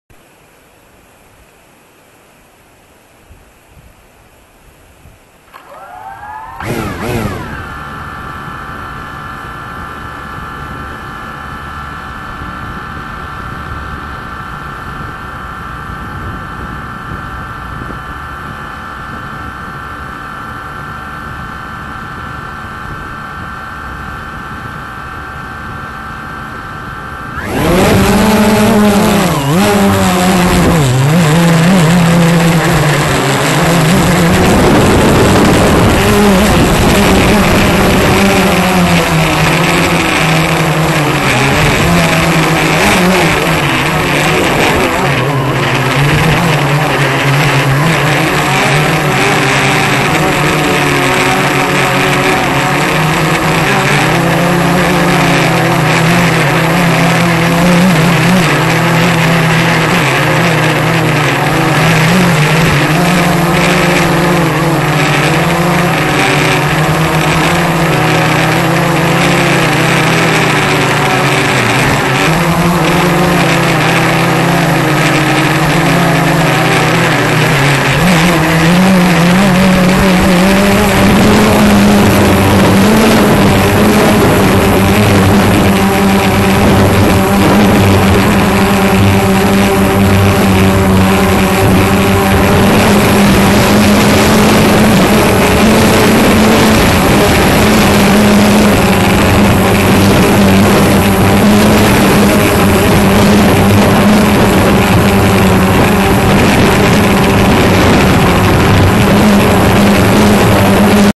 Drone Video 360
Filmagem de drone com câmara 360, na ilha das Flores nos Açores na cascata da bacalhoa